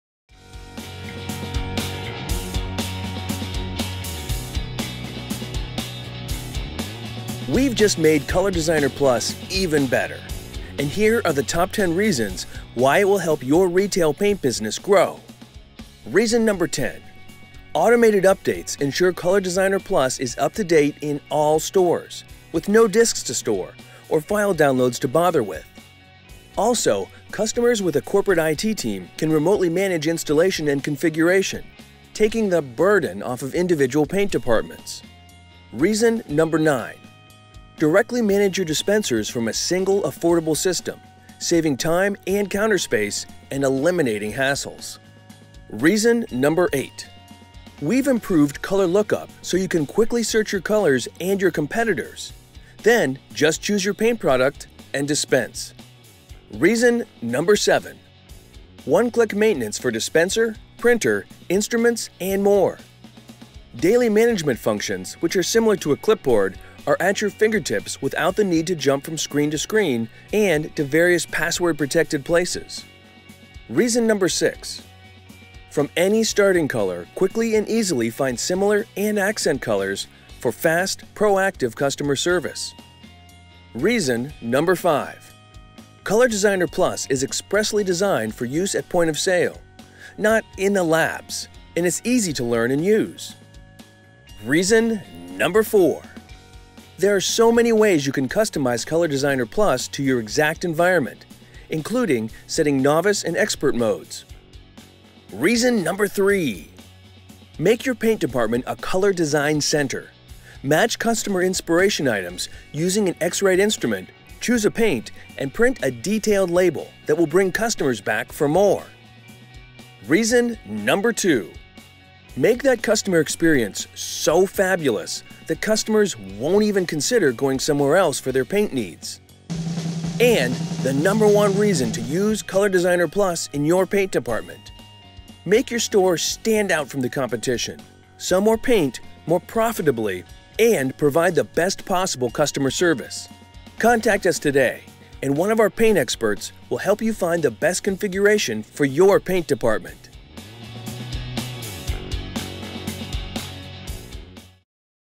ColorDesigner Plus: Top 10 New Features
X-Rite's ColorDesigner® PLUS color matching software expedites and  refines the paint selection process, eliminating wasted effort and  needless formulation errors. It has the ability to connect to a paint  dispenser system for fast delivery of paint color formulations, mix the  customer’s color in the paint product they want and an option to look up  colors across paint lines. With one efficient package, the paint  department becomes a color design center.